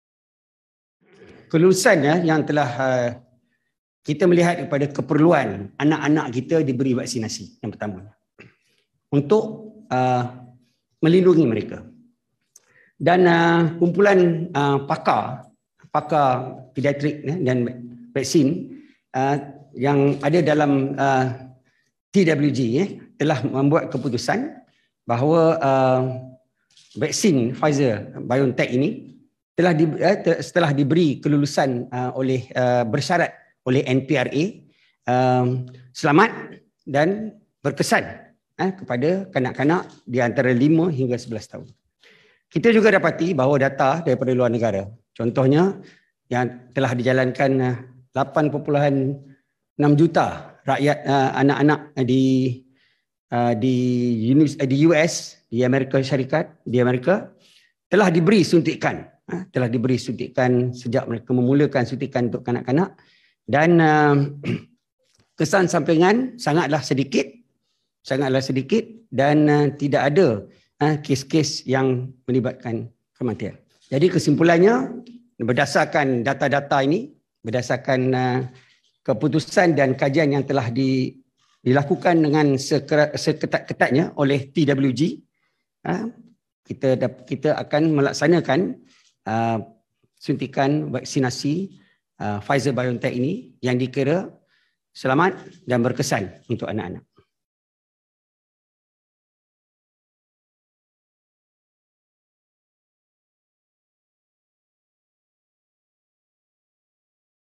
Covid-19: Special vaccine formulation for children is safe, assures Dr Noor Azmi
Deputy Health Minister I Dr Noor Azmi Ghazali, who heads the immunisation programme for children, has assured parents and guardians that the special Pfizer vaccine formulation is safe.